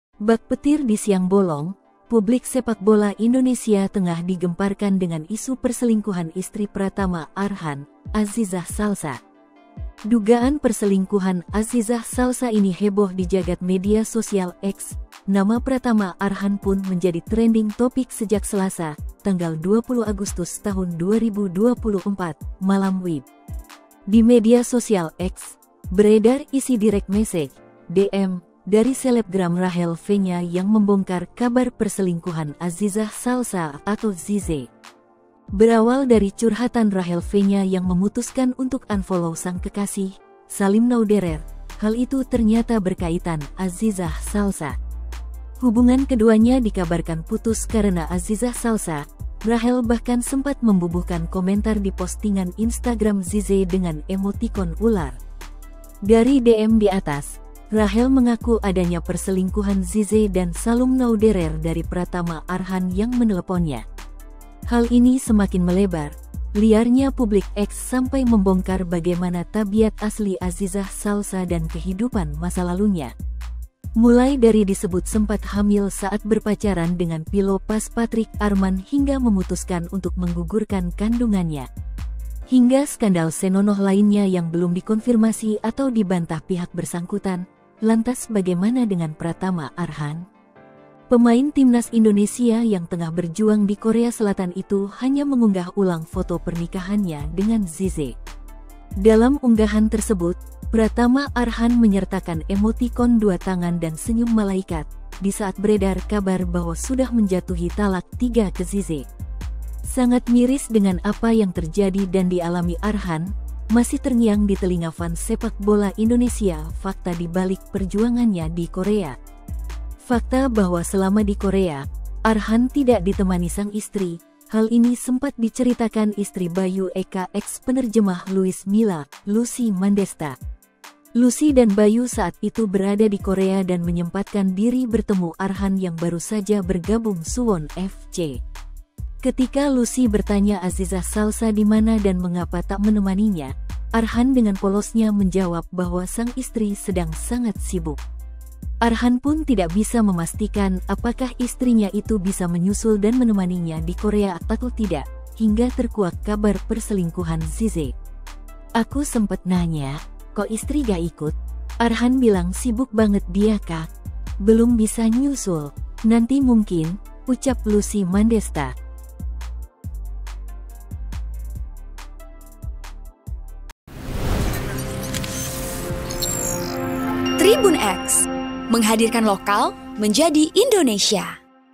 Fakta-Fakta di Balik Isu Selingkuh Azizah Salsha, Jawaban Melas Pratama Arhan Jadi Sorotan
Bak petir di siang bolong, publik sepak bola Indonesia tengah digemparkan dengan isu perselingkuhan istri Pratama Arhan, Azizah Salsha.

Berawal dari curhatan Rachel Vennya yang memutuskan untuk unfollow sang kekasih, Salim Nauderer, hal itu ternyata berkaitan Azizah Salsha.

Hubungan keduanya dikabarkan putus karena Azizah Salsha, Rachel bahkan sempat membubuhkan komentar di postingan Instagram Zize dengan emotikon ular.

Dari DM di atas, Rachel mengaku adanya perselingkuhan Zize dan Salum Nauderer dari Pratama Arhan yang meneleponnya.

Hal ini semakin melebar, liarnya publik X sampai membongkar bagaimana tabiat asli Azizah Salsha dan kehidupan masa lalunya.

Mulai dari disebut sempat hamil saat berpacaran dengan Philo Paz Patric Armand hingga memutuskan untuk menggugurkan kandungannya.

Hingga skandal senonoh lainnya yang belum dikonfirmasi atau dibantah pihak bersangkutan, lantas bagaimana dengan Pratama Arhan?

Pemain Timnas Indonesia yang tengah berjuang di Korea Selatan itu hanya mengunggah ulang foto pernikahannya dengan Zize.

Dalam unggahan tersebut, Pratama Arhan menyertakan emotikon dua tangan dan senyum malaikat, di saat beredar kabar bahwa sudah menjatuhi talak 3 ke Zize.

Sangat miris dengan apa yang terjadi dan dialami Arhan, masih terngiang di telinga fan sepak bola Indonesia fakta di balik perjuangannya di Korea.

Fakta bahwa selama di Korea, Arhan tidak ditemani sang istri, hal ini sempat diceritakan istri Bayu Eka eks penerjemah Luis Milla, Lucy Mandezta.

Lucy dan Bayu saat itu berada di Korea dan menyempatkan diri bertemu Arhan yang baru saja bergabung Suwon FC.

Ketika Lucy bertanya Azizah Salsha di mana dan mengapa tak menemaninya, Arhan dengan polosnya menjawab bahwa sang istri sedang sangat sibuk.

Arhan pun tidak bisa memastikan apakah istrinya itu bisa menyusul dan menemaninya di Korea atau tidak, hingga terkuak kabar perselingkuhan Zize.

"Aku sempet nanya, kok istri gak ikut? Arhan bilang sibuk banget dia kak. Belum bisa nyusul. Nanti mungkin," ucap Lucy Mandezta.

Program: -
Host: -
Editor Video: Gabriel
Uploader: -